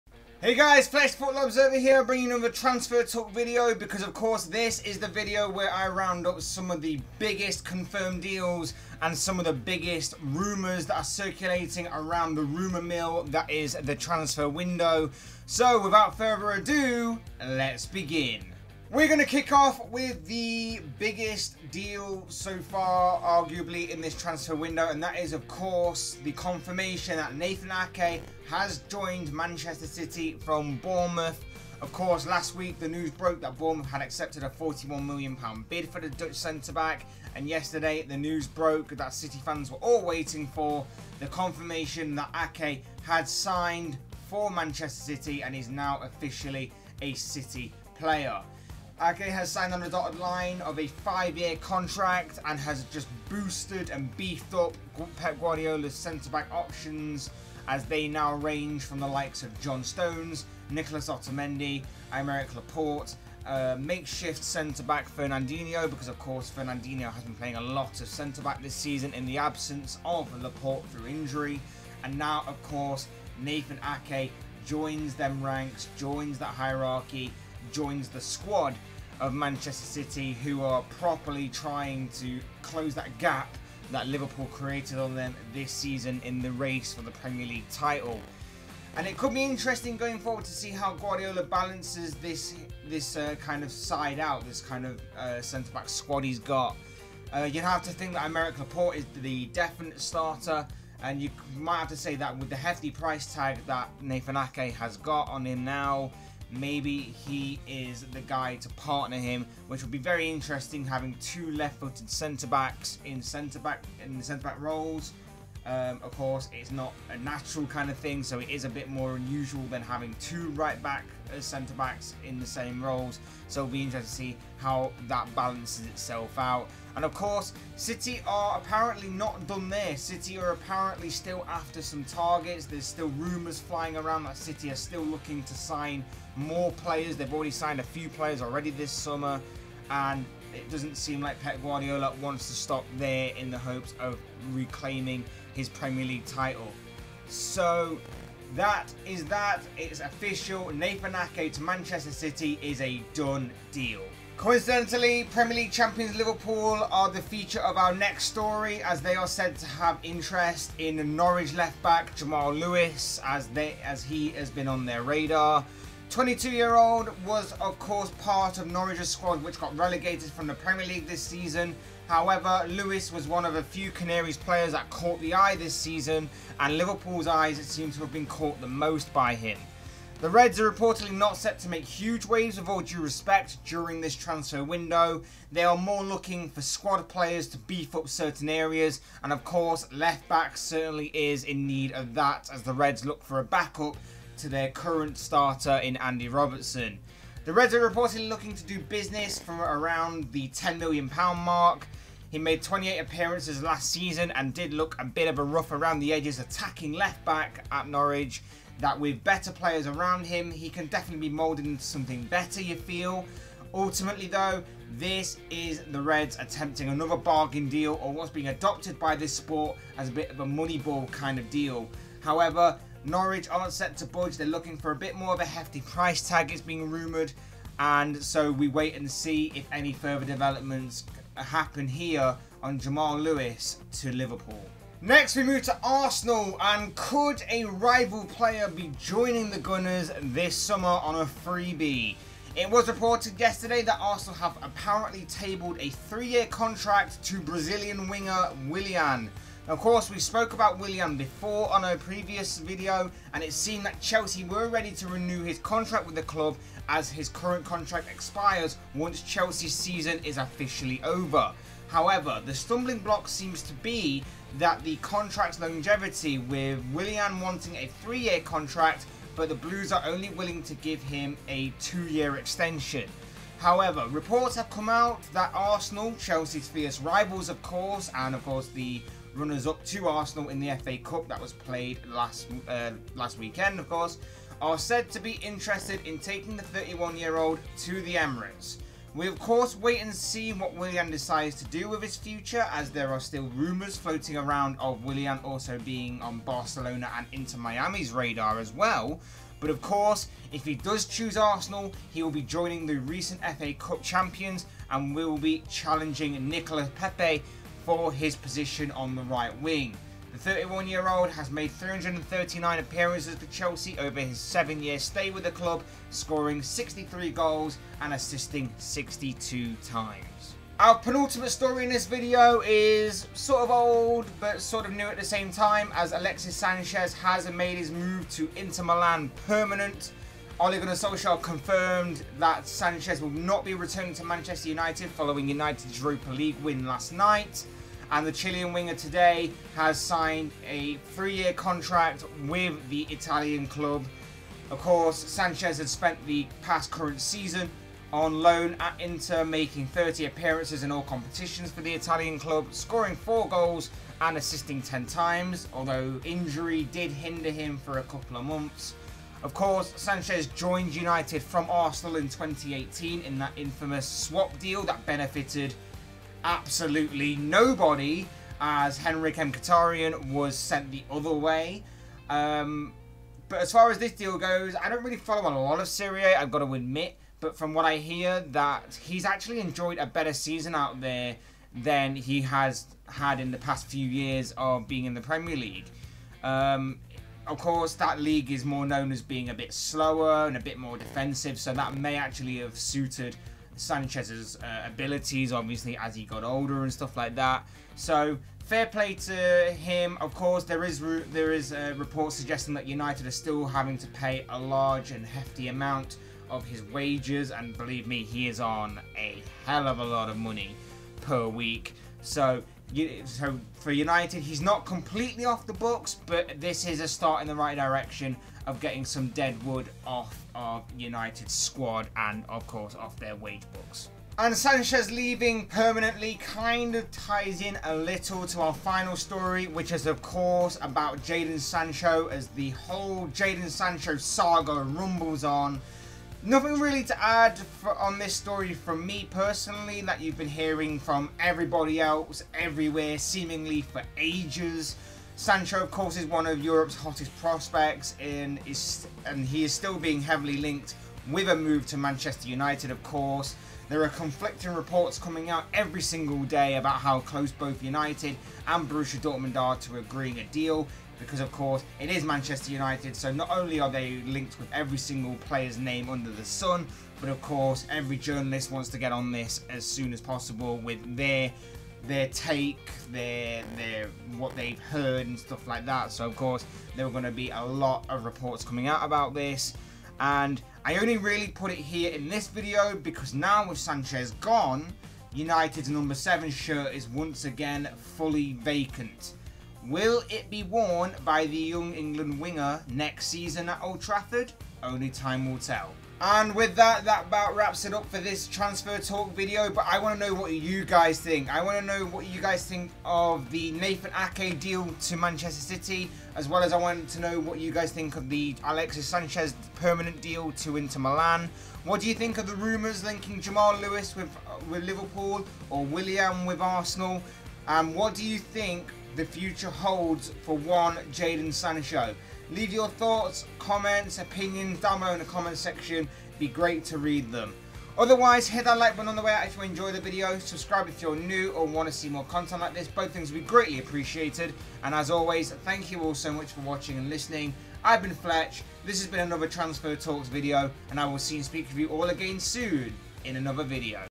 Transfer Talk: Inter Permanently Sign Sanchez!
Another video of Transfer Talk is upon us and this video features the confirmation of Nathan Ake joining Manchester City, Jamal Lewis attracting interest from Liverpool, Arsenal in pole position to sign a Chelsea player, Inter Milan permanently signing Alexis Sanchez, and a tiny bit on Jadon Sancho too.